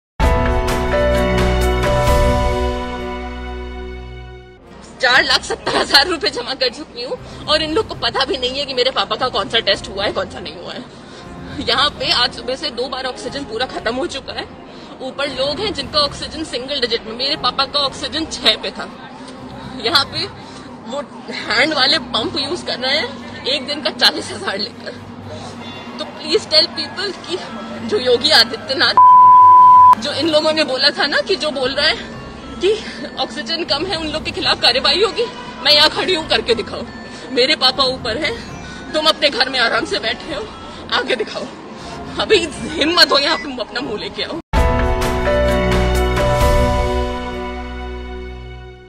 बेटी ने दिया सीएम योगी को खुला चैलेंज !| News Time Nation
मजलूम लड़की ने यूपी सरकार को किया चैलेंज
अस्पताल में नहीं है ऑक्सीजन
ऑक्सीजन की कमी से तड़प रहे मरीज
यूपी सरकार से नाराज है लड़की